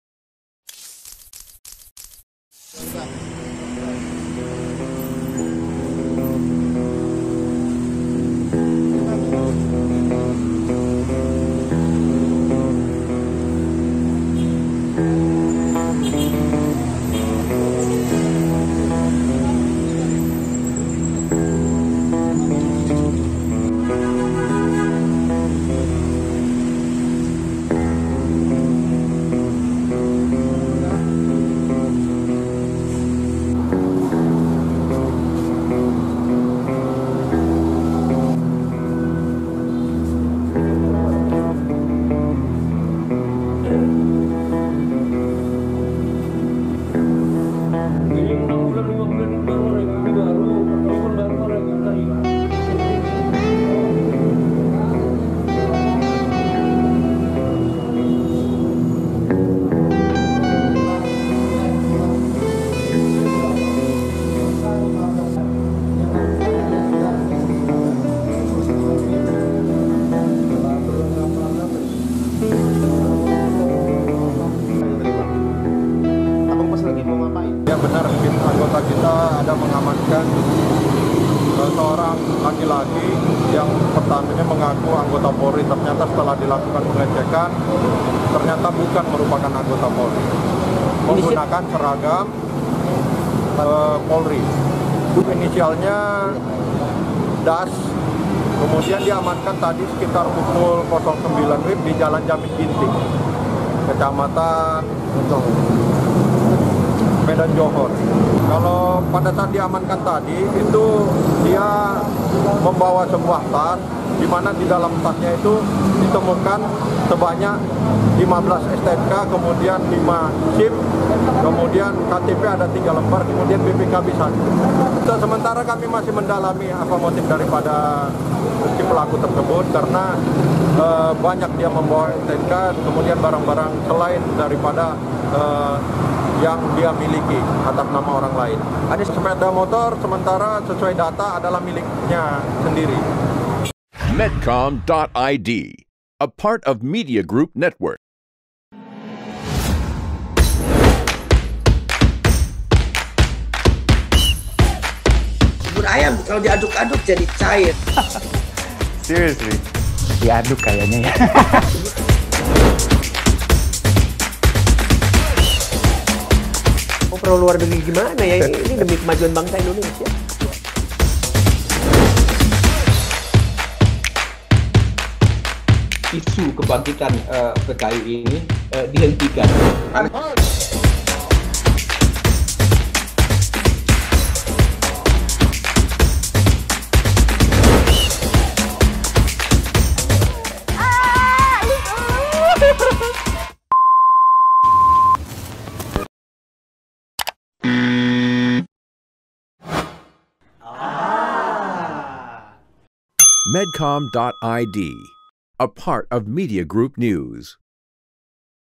Ngaku Jadi Polantas, Polisi Gadungan di Medan Diringkus Polisi
Diduga kerap memeras pengendara, seorang polisi gadungan berinisial DAS yang berpura-pura sebagai polisi lalu lintas diamankan personel Polsek Delitua, Kota Medan, Sumatera Utara, pada Selasa (3/5/22).

Pada awalnya, personel mencurigai gerak gerik pelaku sehingga mengintrogasinya. Benar saja, saat diamankan dan dilakukan pemeriksaan, pelaku tidak dapat menunjukkan Kartu Tanda Anggota (KTA) Polri.
Pelaku mengaku sudah satu tahun menjadi polisi gadungan dan kerap menilang pengendara. Dari tangan pelaku, polisi juga menemukan belasan STNK dan SIM serta satu unit borgol dan alat komunikasi HT.

Kapolsek Delitua, Kompol Dedy Dharma, mengatakan pihaknya saat ini tengah melakukan pendalaman dan penyelidikan terhadap pelaku polisi gadungan.